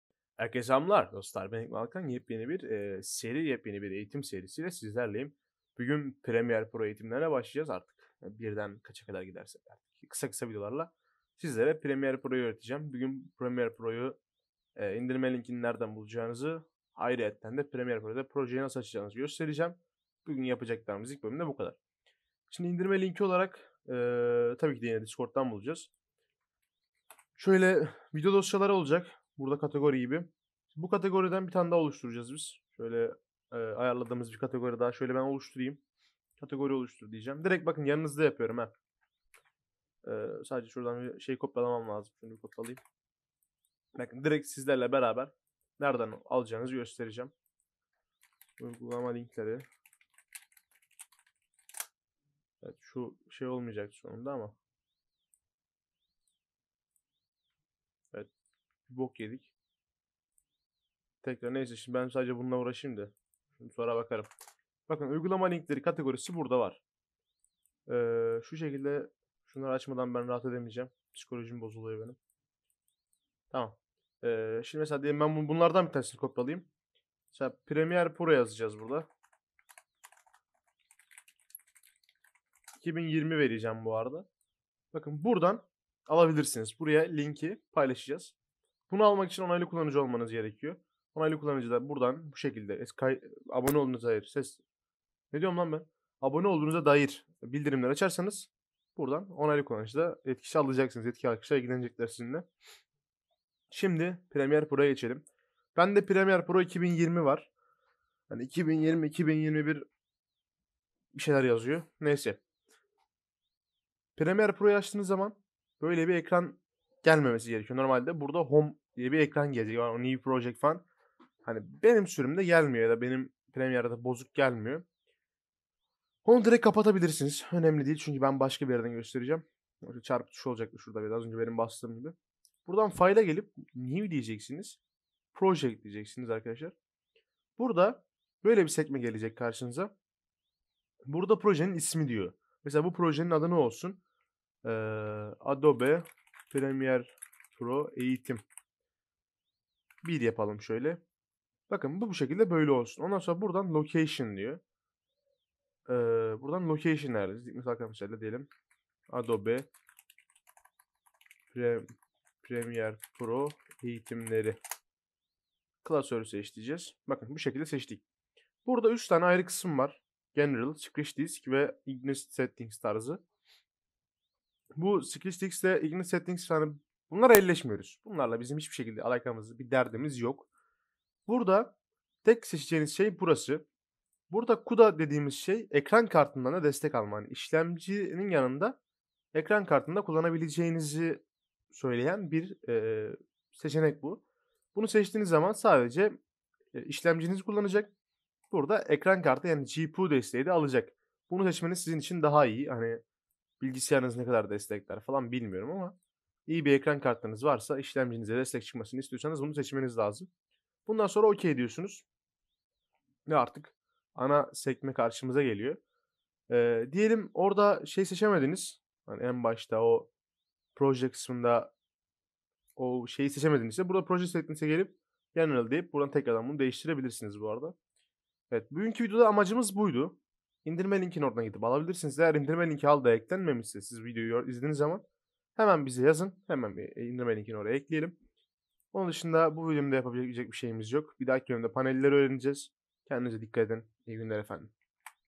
#1 - Nasıl İndirilir ? * Nasıl Proje Oluşturulur ? | Adobe Premier Pro Eğitim Serisi
✅ Merhabalar, dostlar bugün premier pro eğitim serisi yayınladım iyi seyirler dilerim bir sorununuz olursa discorda gelebilirsiniz.

💥 İNDİRME LİNKLERİ DİSCORD'DA

Etiketler;